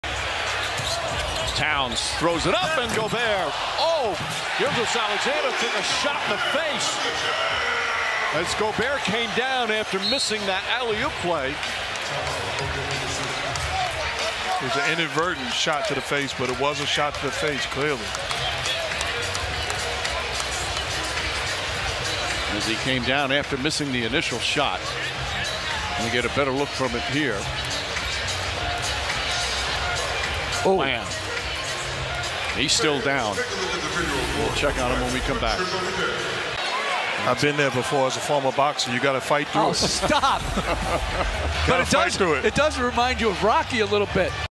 😳 SGA NASTY CUT & BLACK EYE after Elbow in face by Rudy Gobert
In this YouTube Shai Gilgeous-Alexander takes an elbow to the face by Rudy Gobert in the play-in game where the Minnesota Timberwolves defeat the Oklahoma City Thunder.